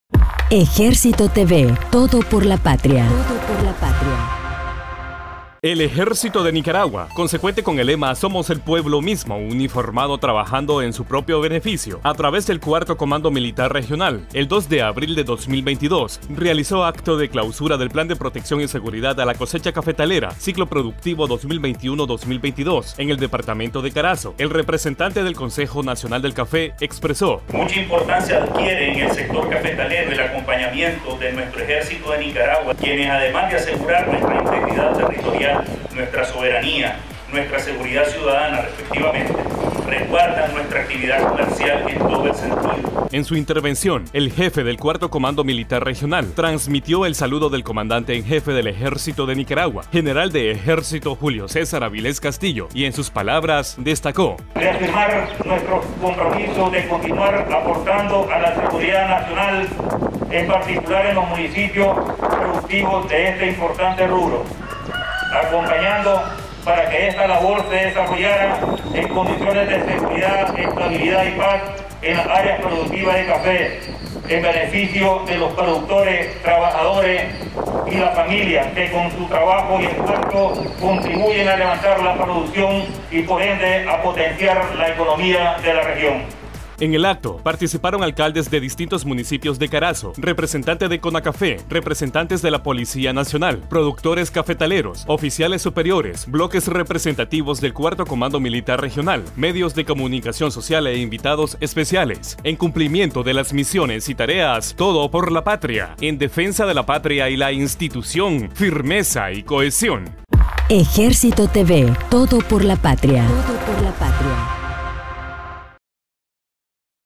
Clausura del Plan de Protección y Seguridad a la Cosecha Cafetalera en Carazo
El 2 de abril de 2022, a través del 4 Comando Militar Regional, se realizó acto de Clausura del Plan de Protección y Seguridad a la Cosecha Cafetalera, ciclo productivo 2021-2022, en el departamento de Carazo.